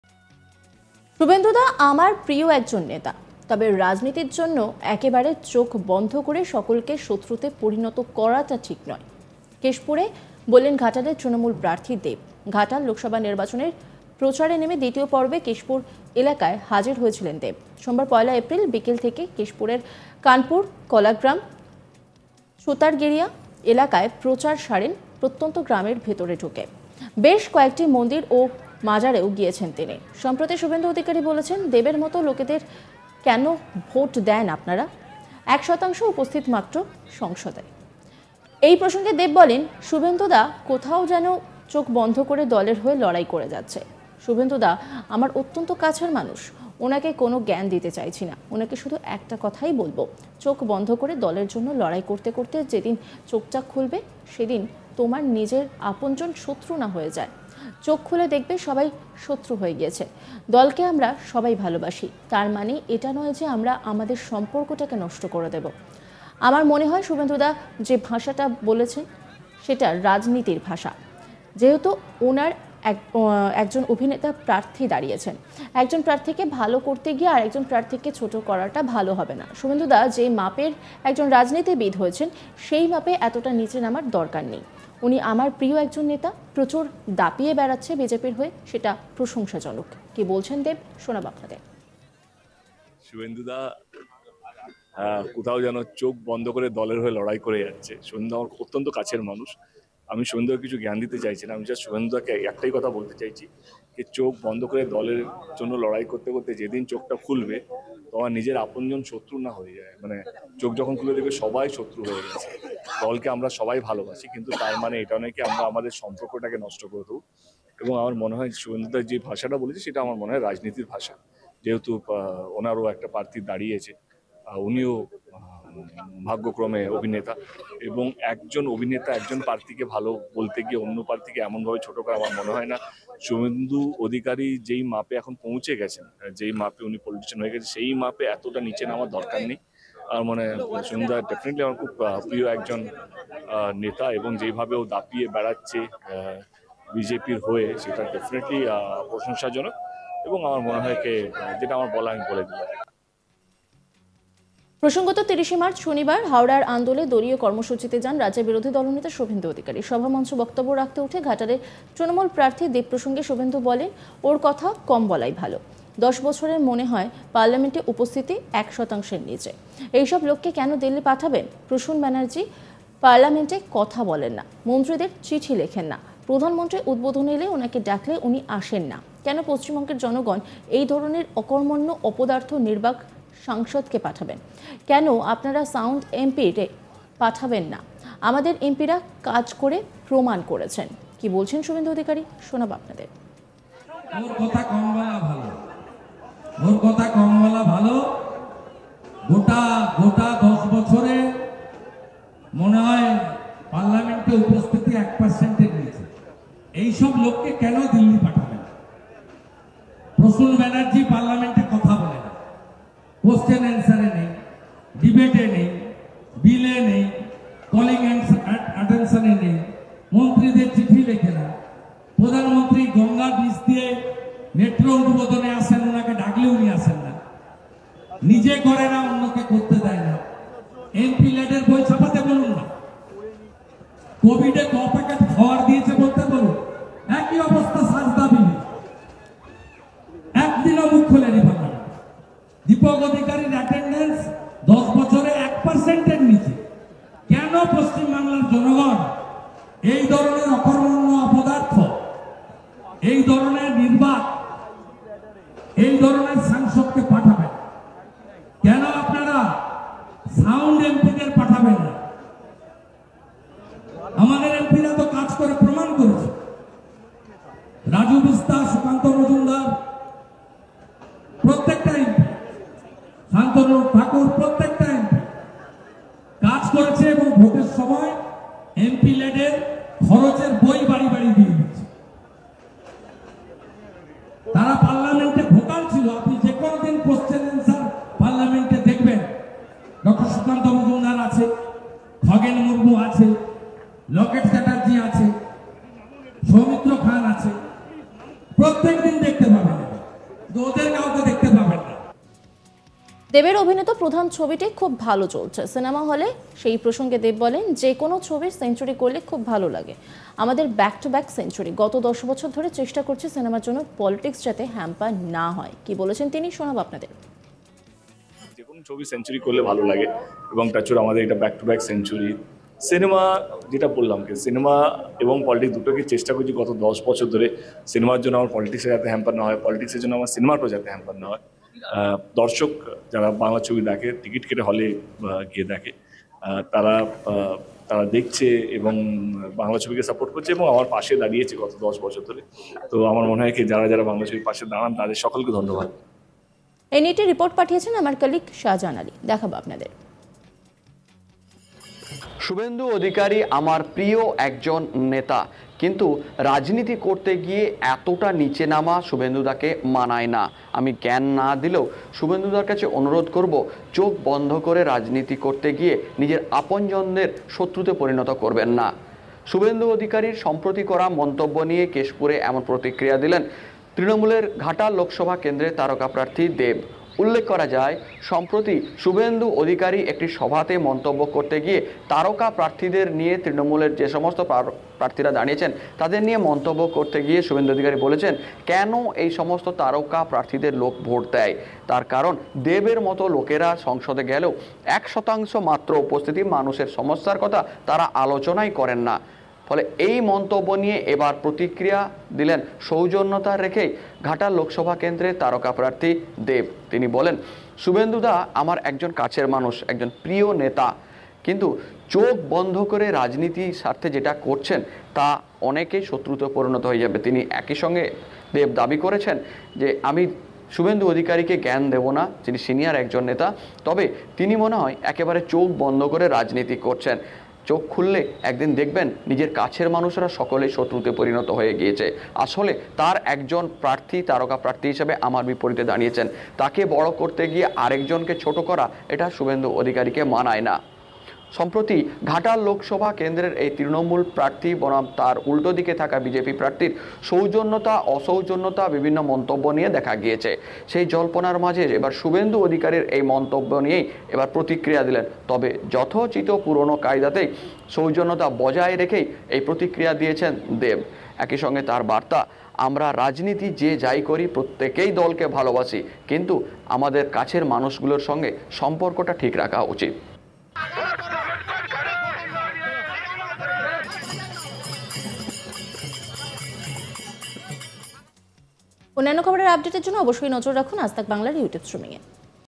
Breaking: 'শুভেন্দুদা অত্যন্ত কাছের মানুষ', দেব মুখ ফস্কে বললেন না হিসেব করে? Dev | Suvendu Adhikari
"শুভেন্দু দা আমার প্রিয় একজন নেতা, তবে রাজনীতির জন্য একেবারে চোখ বন্ধ করে সকলকে শত্রুতে পরিণত করাটা ঠিক নয়।"-কেশপুরে বললেন ঘাটালের তৃণমূল প্রার্থী দেব। ঘাটাল লোকসভা নির্বাচনের প্রচার নেমে দ্বিতীয় পর্বে কেশপুর এলাকায় হাজির হয়েছিলেন দেব। সোমবার, ১ এপ্রিল বিকেল থেকে কেশপুরের কানপুর,কলাগ্রাম ,ছুতারগেড়িয়া এলাকায় প্রচার সারেন প্রত্যন্ত গ্রামের ভেতরে ঢুকে। বেশ কয়েকটি মন্দির ও মাজারেও গিয়েছেন তিনি। সম্প্রতি শুভেন্দু অধিকারী বলেছেন-"দেবের মতো লোকেদের কেন ভোট দেন আপনারা? এক শতাংশ উপস্থিত মাত্র সংসদে।" এই প্রসঙ্গে দেব বলেন-"শুভেন্দু দা কোথাও যেন চোখ বন্ধ করে দলের হয়ে লড়াই করে যাচ্ছে।